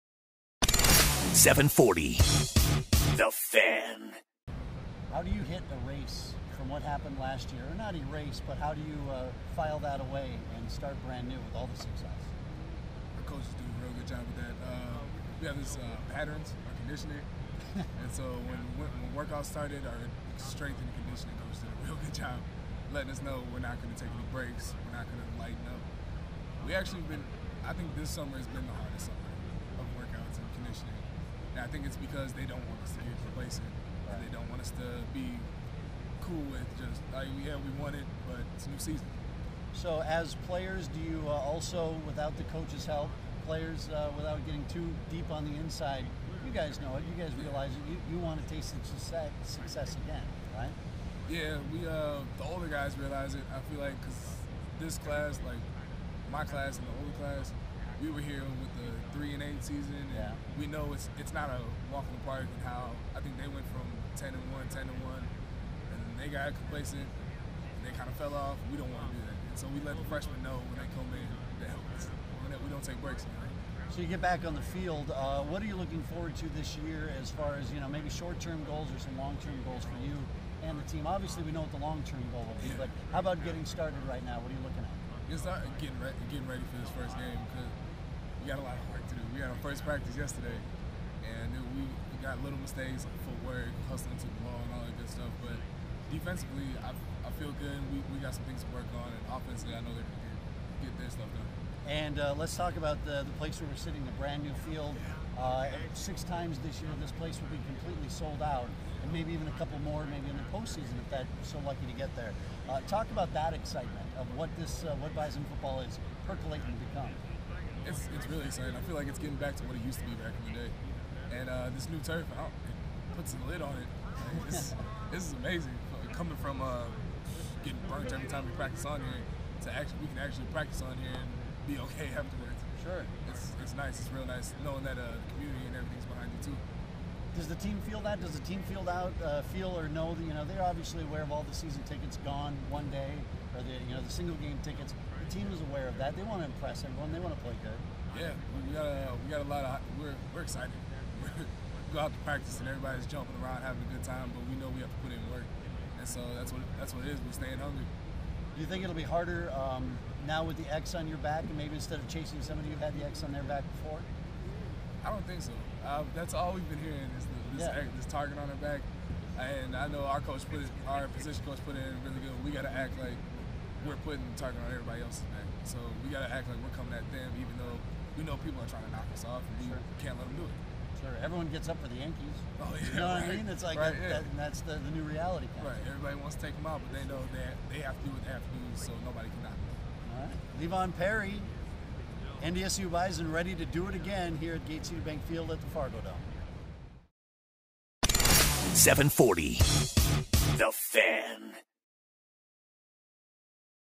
740TheFan: Chris Hanson With Bison DT Leevon Perry
740TheFan's Chris Hanson sits down and talks with NDSU Bison defensive tackle Leevon Perry.